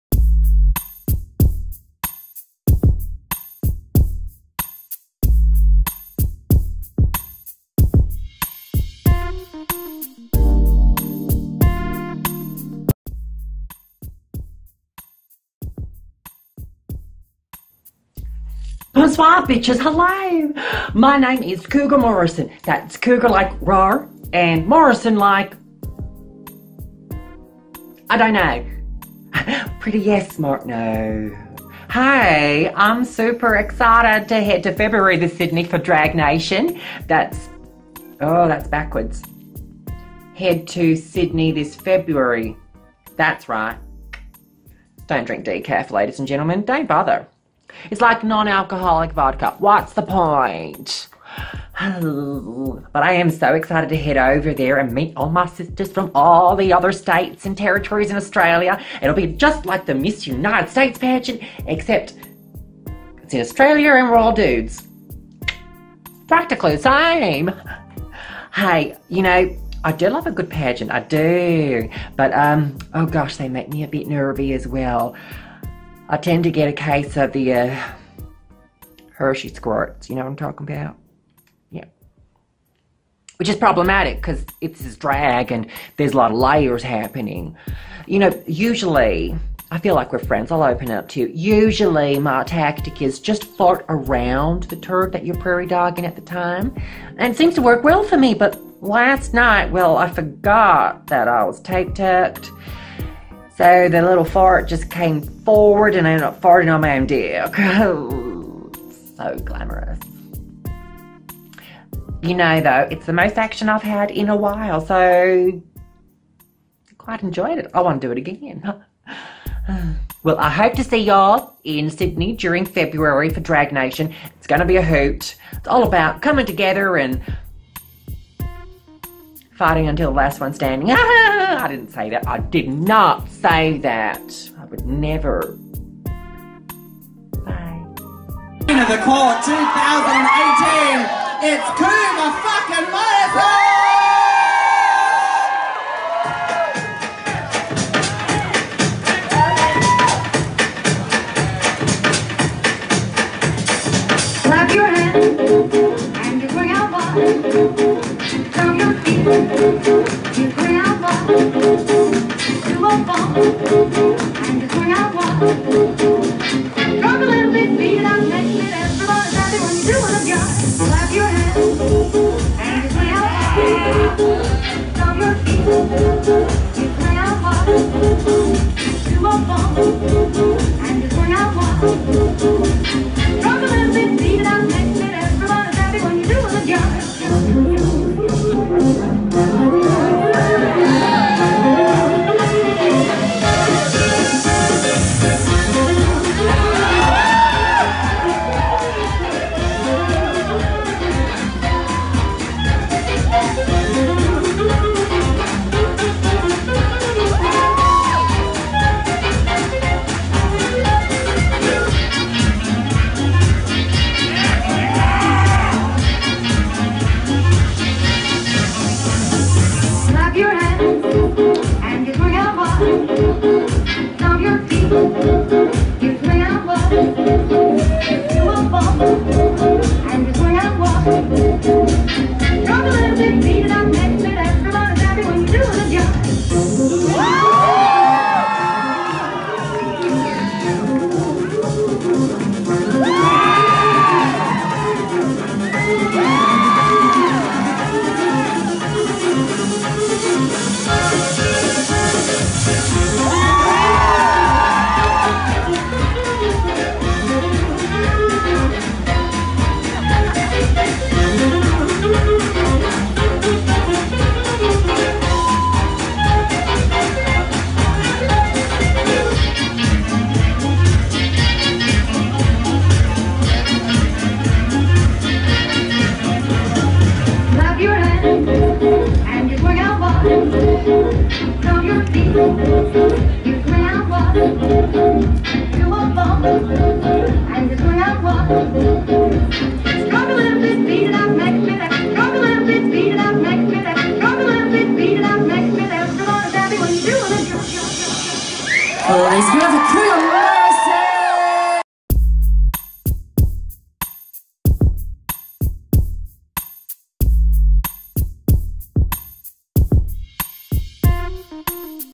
Dragnation WA Cougar Morrison
From The Court in Perth Western Australia comes winner of Queen of the Court- Cougar Morrison!